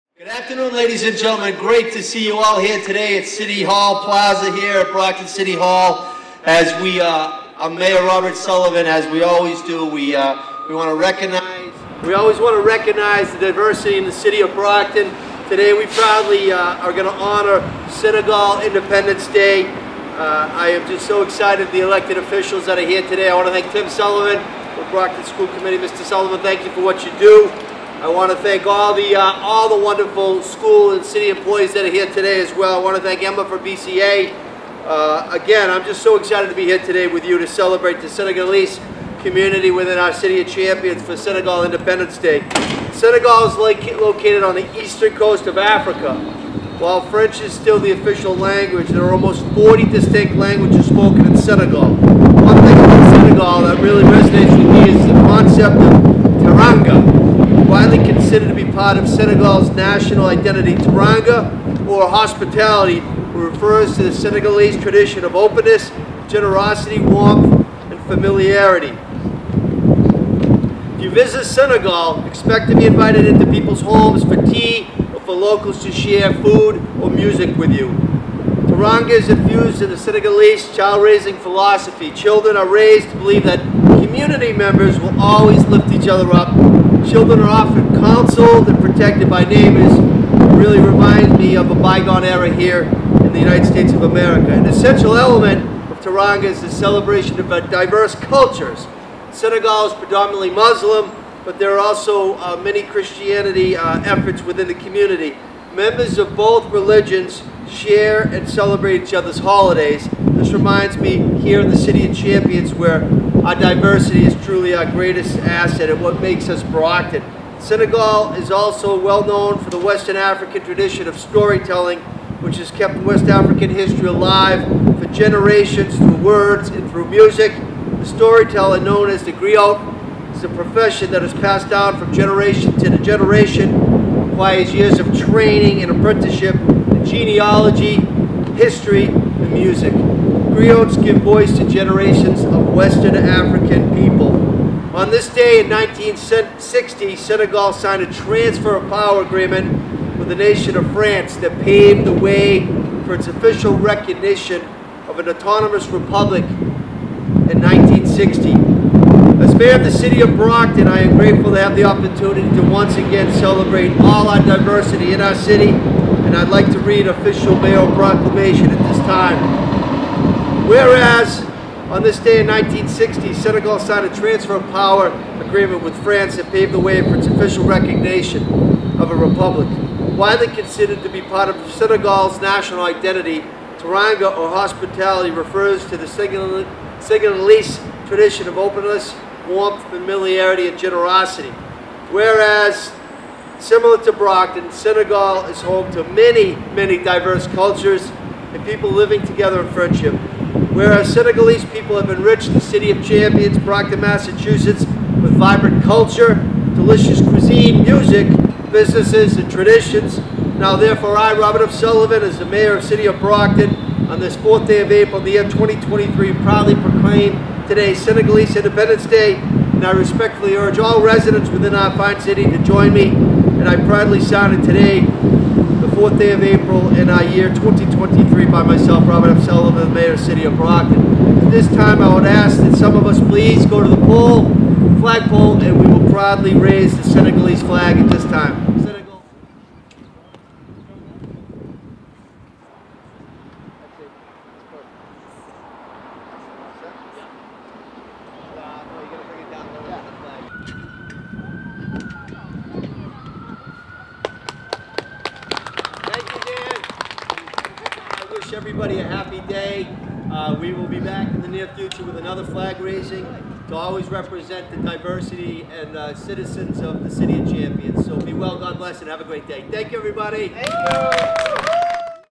Senegalese Flag Raising 4-4-23
Mayor Robert Sullivan celebrates Senegal Independence Day in the City of Brockton.